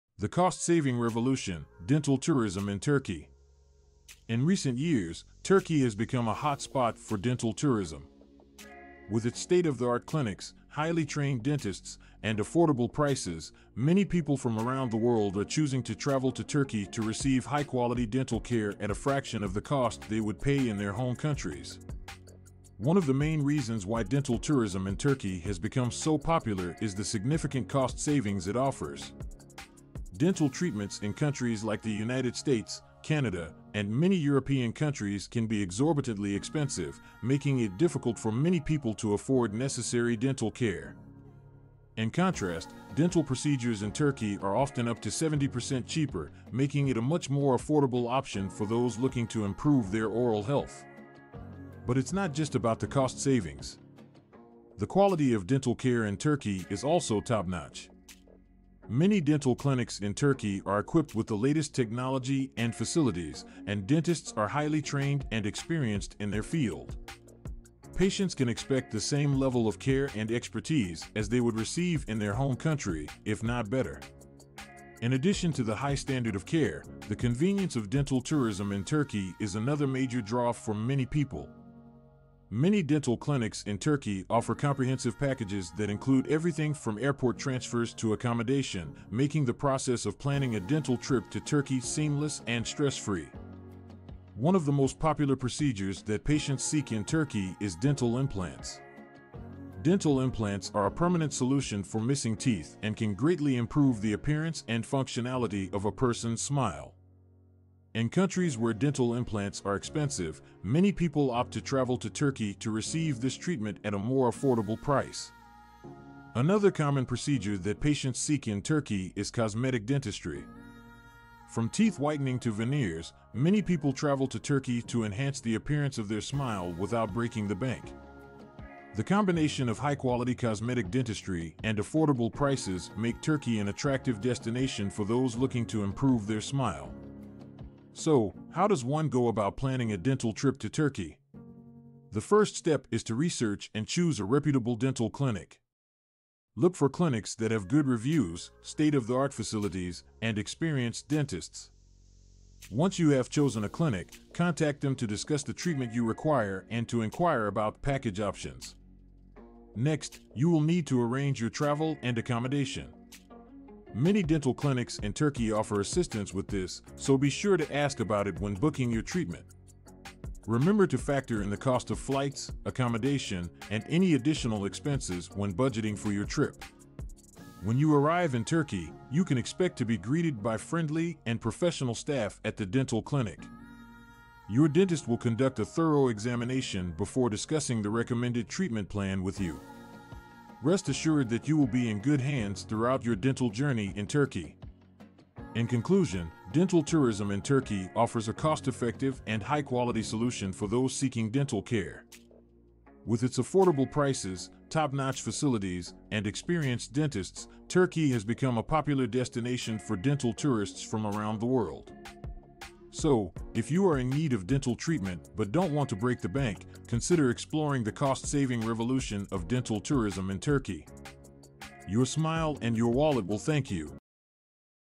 The Cost-Saving Revolution: Dental Tourism in Turkey - Is it the Best Option for Your Next Dental
Telegram Turkey for their dental treatments. Dental tourism in Turkey offers significant cost savings compared to other countries, with procedures costing up to 70% less. This has led to a cost-saving revolution, where people are traveling to Turkey specifically for their dental needs. The country's strategic location, cultural attractions, and easy visa process also make it a desirable destination for dental tourists. With a wide range of services offered, including dental implants, veneers, and whitening treatments, Turkey has established itself as a top destination for those seeking high-quality and affordable dental care.